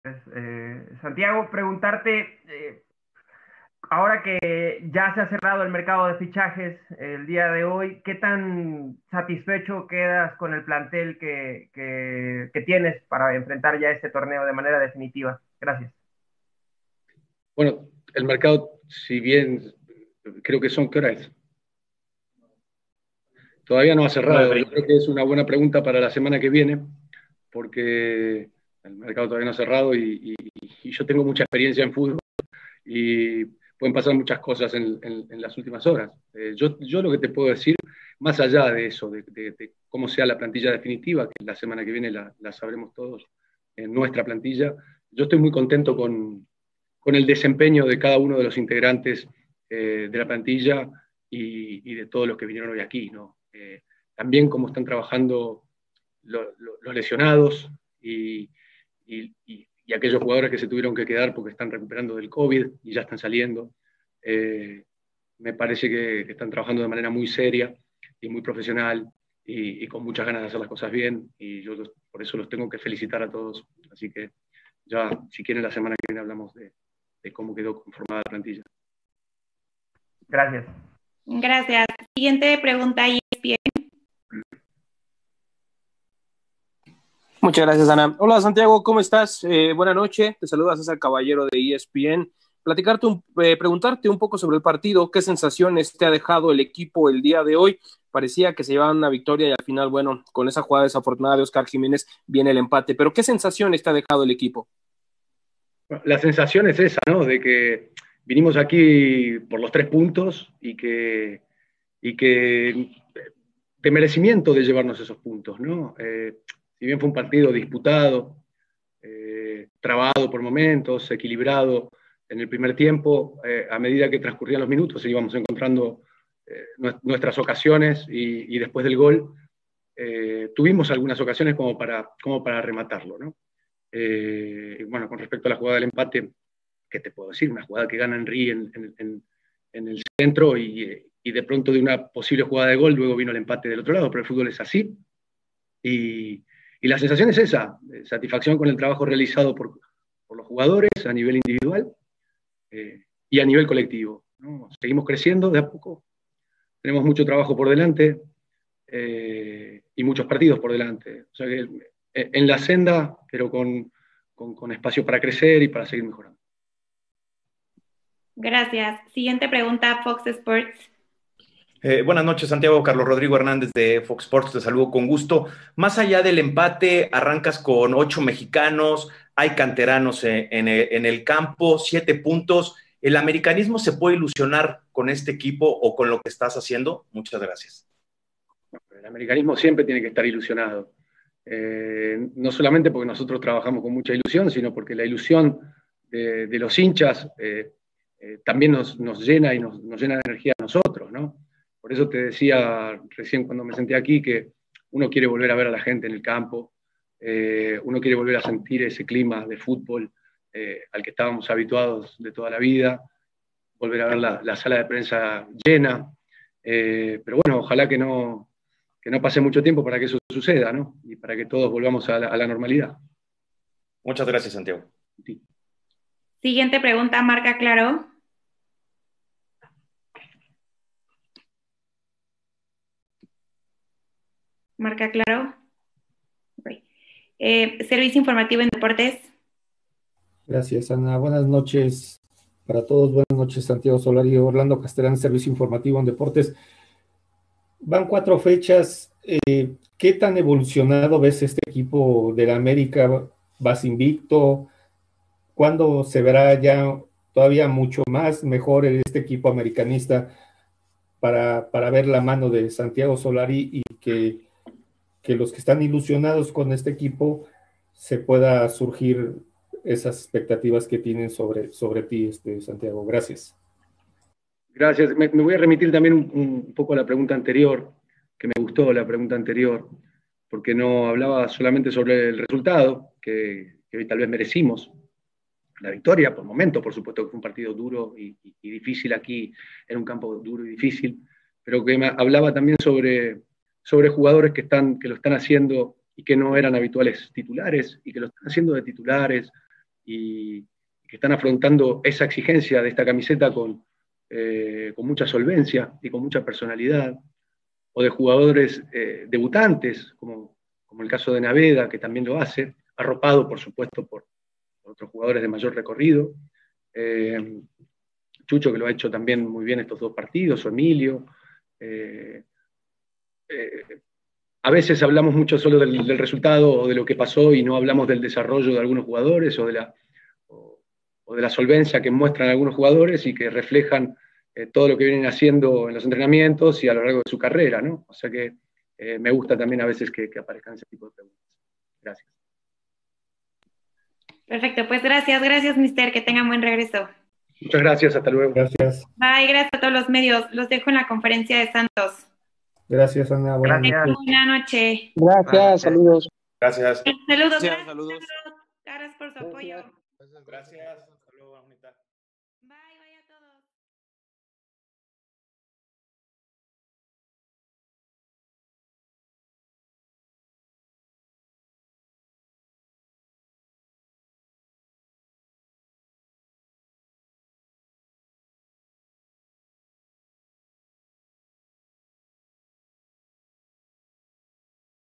SantiagoSolari habla después del empate de América vs
Molestia por perder dos puntos